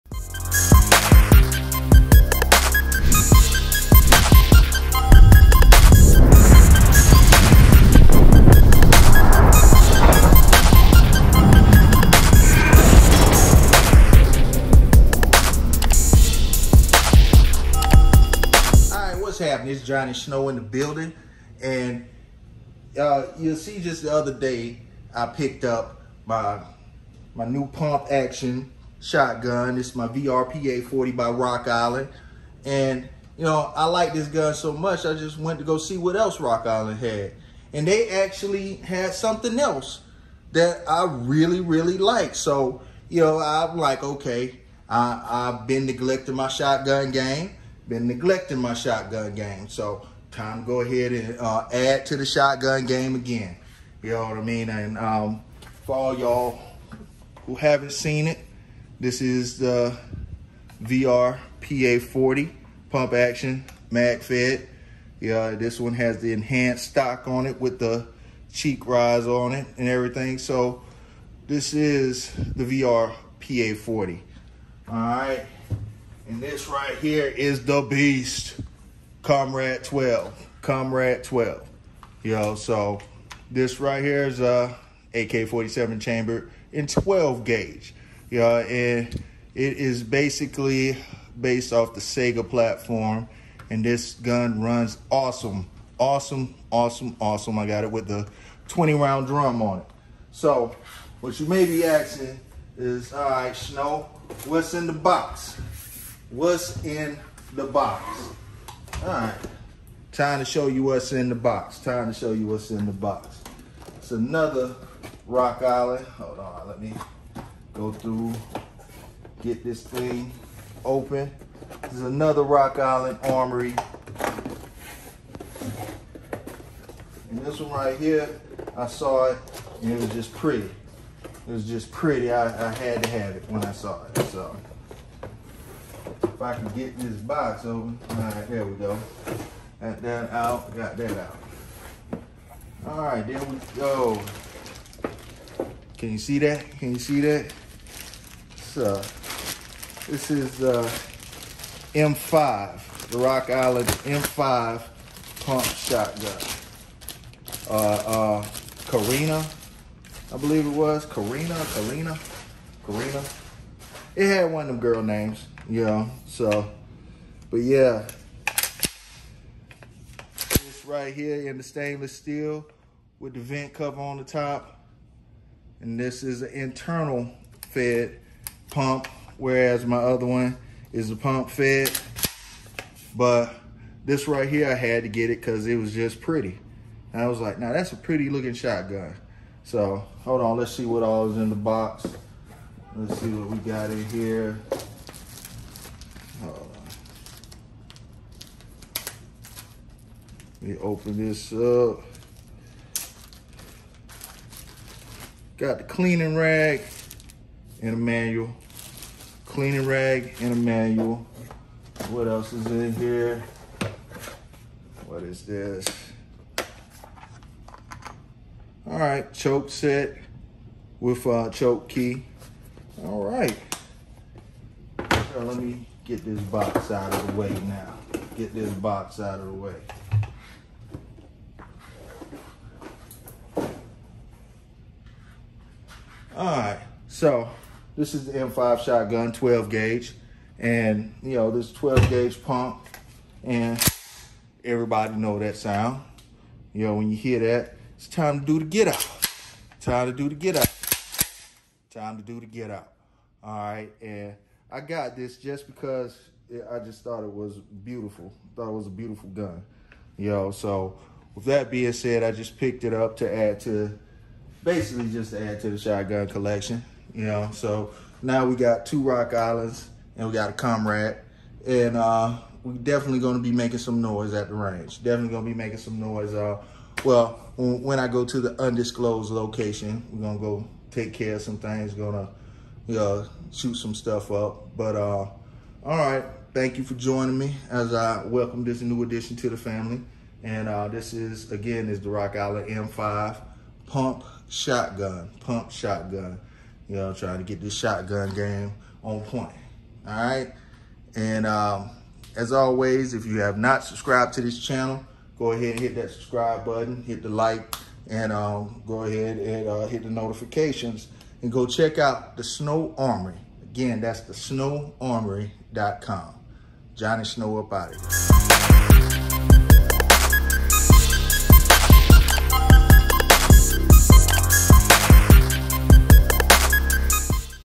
Adding To My Shotgun Collection Unboxing My Rock Island Armory Carina M5 Pump Shotgun
Adding a Carina M5 By Rock Island Armory to my Shotgun collection. This is the initial unboxing review and my first time holding the weapon. I just purchased another Rock Island Pump Shotgun the other day The VRPA40.